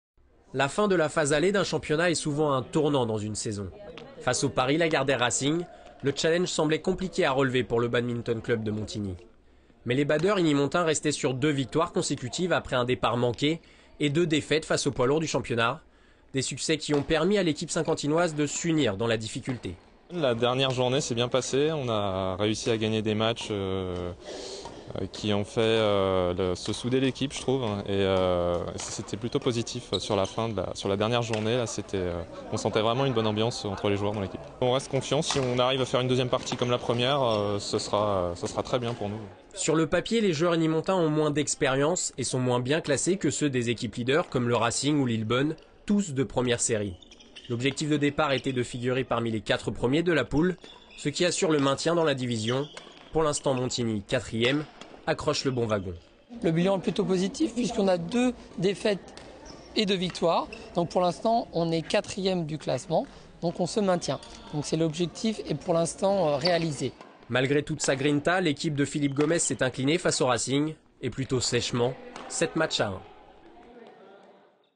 L'Actu - Montigny chute en badminton
En badminton, nationale 3, Montigny, 4e, s'est logiquement incliné face au second de la poule, Paris Lagardère Racing, 7 sets à 1. Le match se disputait au gymnase Jean-Maréchal.